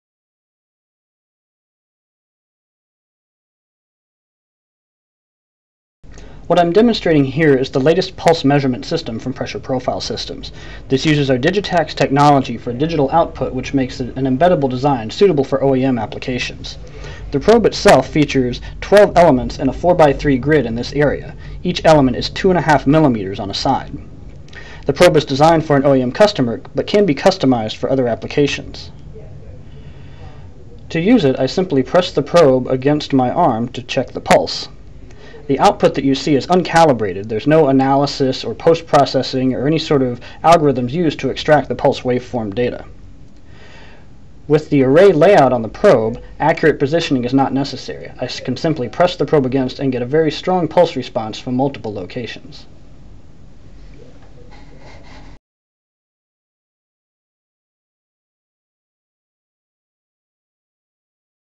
PPS' Tactile Sensor for Pulse Waveform Measurement
Demo and overview of PPS's latest pulse waveform measurement system, featuring digital output and an OEM-capable design.